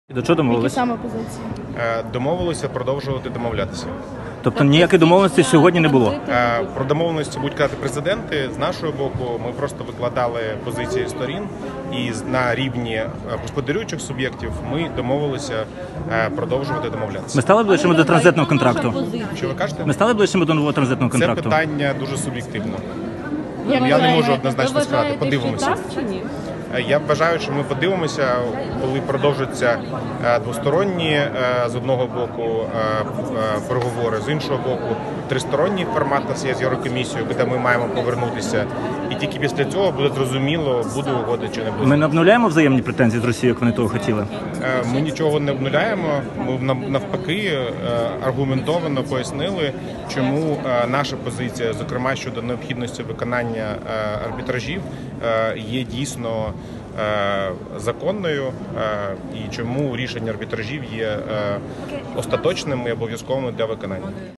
Юрій Вітренко у Парижі: "Домовилися продовжувати домовлятися"
Виконавчий директор Нафтогазу Юрій Вітренко відповів на запитання кореспондента ВВС щодо прогресу в газових питаннях на перемовинах у Парижі.

Нормандська зустріч: а що вирішили з газом?

 Нормандські переговори відбулися у Франції за участі Володимира Зеленського, Володимира Путіна, Ангели Меркель та Емманюеля Макрона.

У складі української та російської делегацій були і представники "Нафтогазу" і "Газпрому"